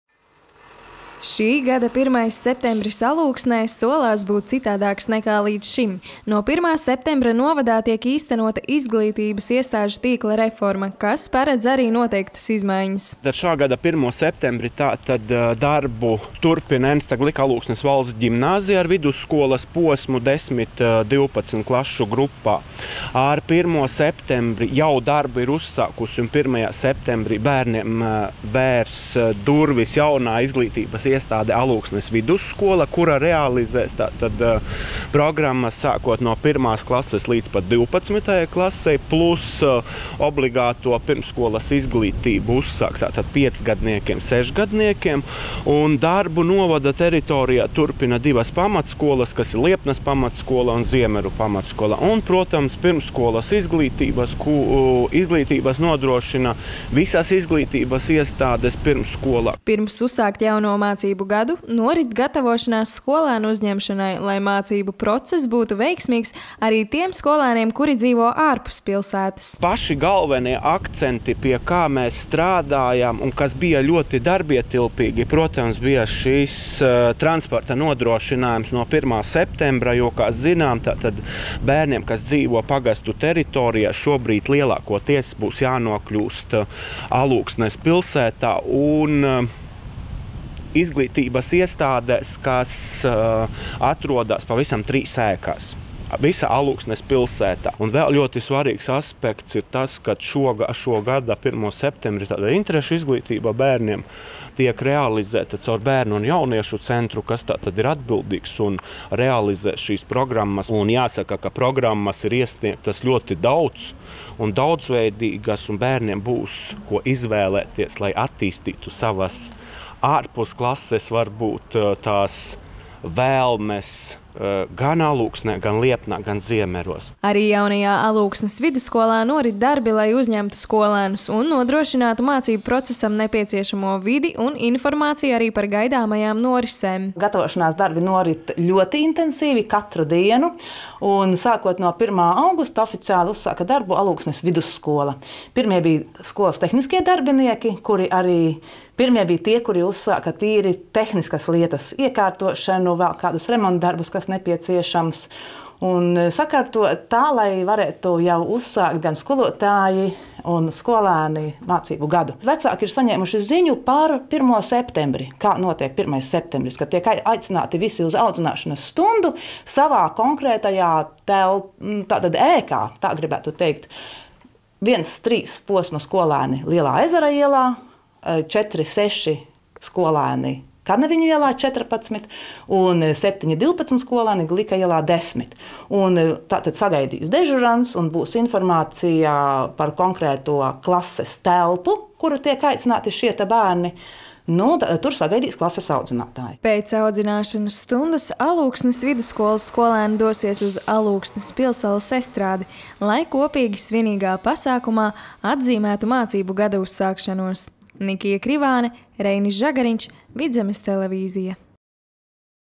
Jaunais mācību gads Alūksnes novadā
Šis jaunais mācību gads būs īpašs gan katram pirmklasniekam, kas šogad sāks skolas gaitas, gan visiem bērniem un jauniešiem, kas sāks mācīties jaunajā izglītības iestādē - Alūksnes vidusskolā.
Pašvaldības izpilddirektors Ingus Berkulis un Alūksnes vidusskolas direktore Ilze Līviņa stāsta par paveikto, gatavojoties šim mācību gadam un skolēnu uzņemšanai.